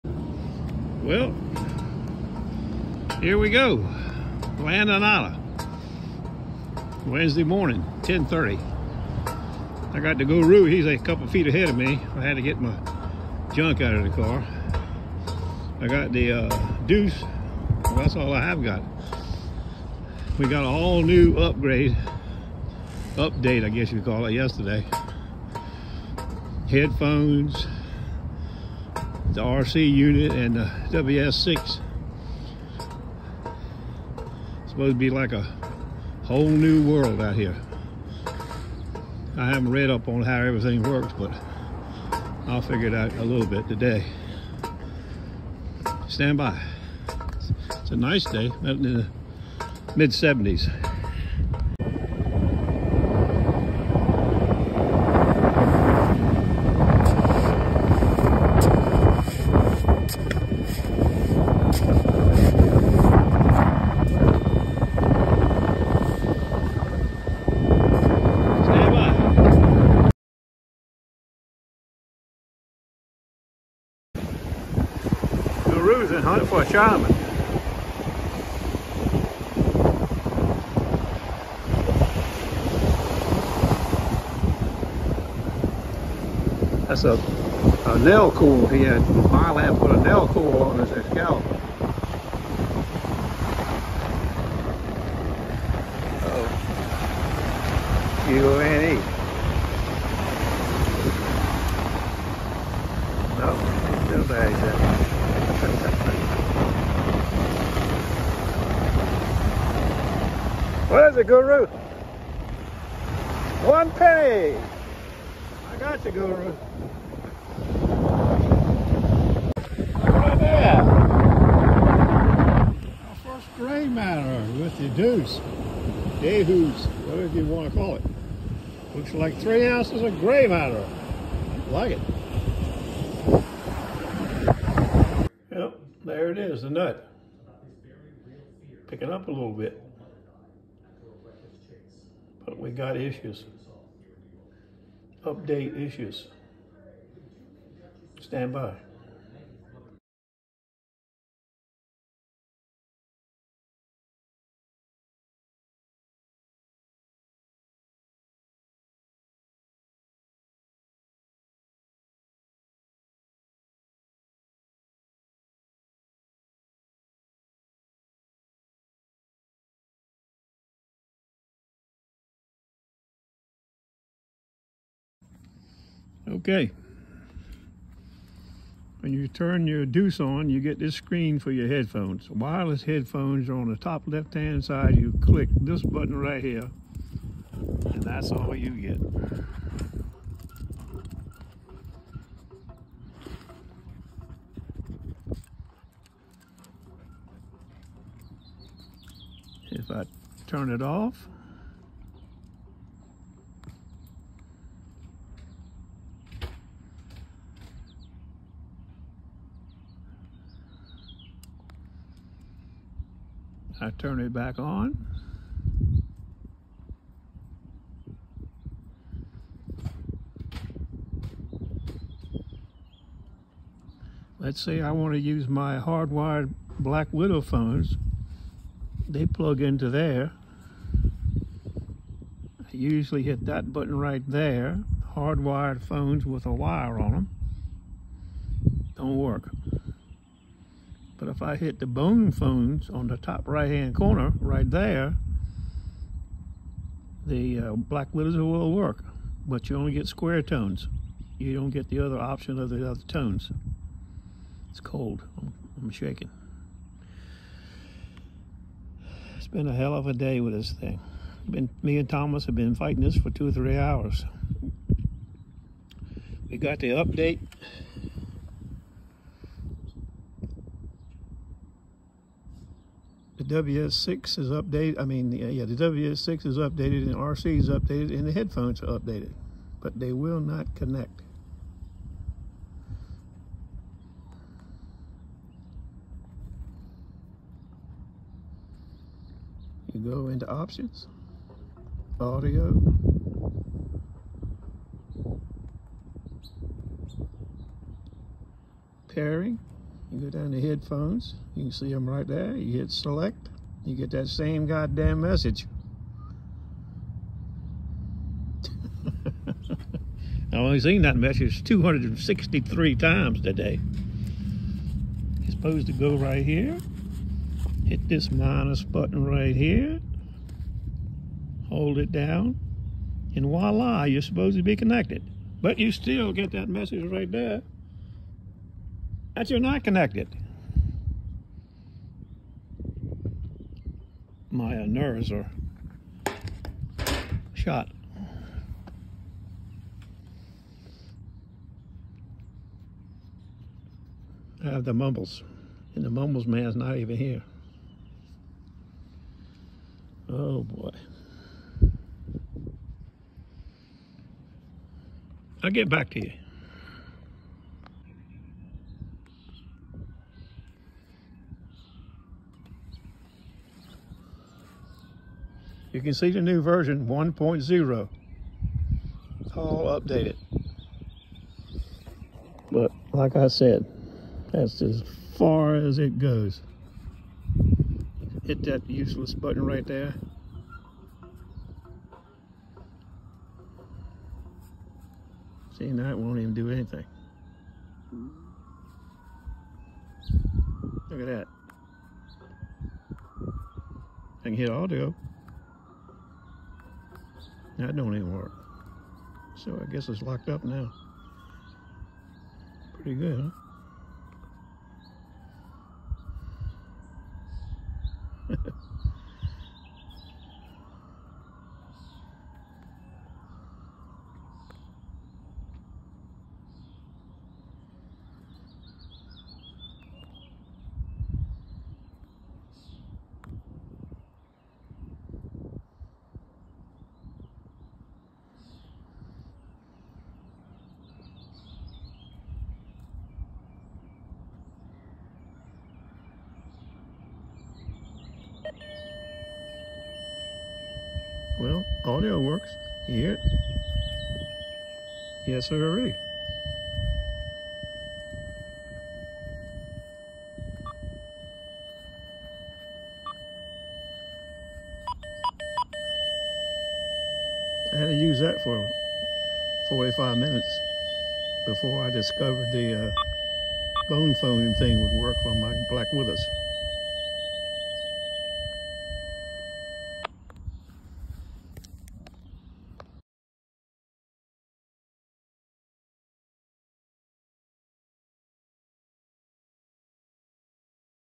XP DeusII - Nada....S/S chain w/2 keys
kinda windy, rather cold for us two today.

deus new updates screwed up a few things. hopefully they will be resolved soon.

Mumblesman gold TOO Nada